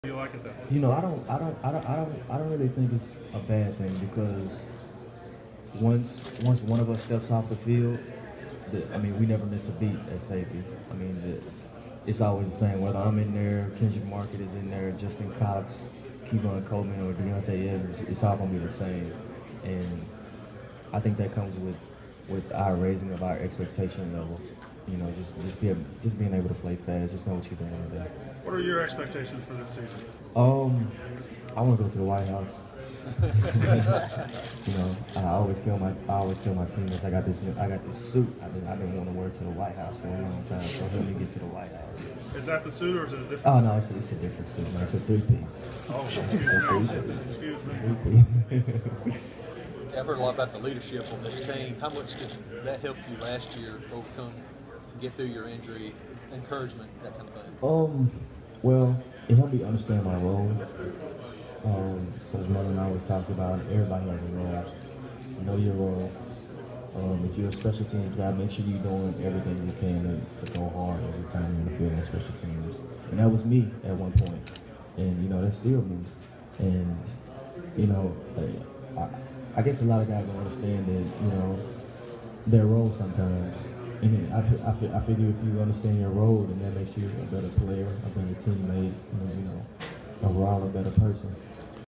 Mississippi State defensive back Jay Hughes.
Jay Hughes talks expectations for the Bulldogs.